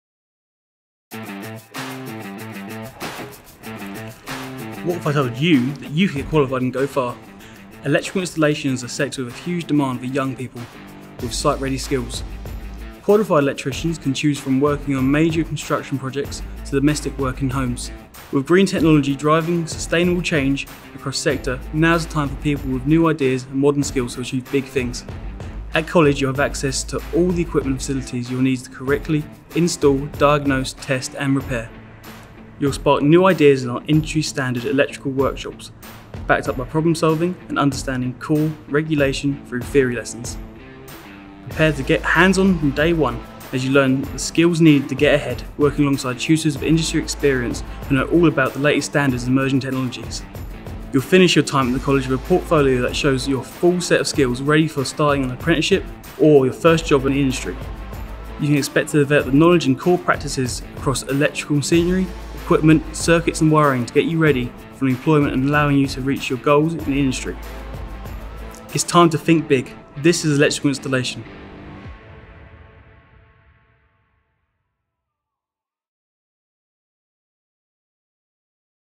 This is Electrical Installation
What if I told you that you can get qualified and go far. Electrical Installation is a sector that has a huge demand for young people with site-ready skills, qualified electricians can choose from working on major construction projects to domestic work in homes.

With green technology driving sustainable change across the sector, now is the time for people with new ideas and modern skills to achieve big things. It's time to think big and apply today.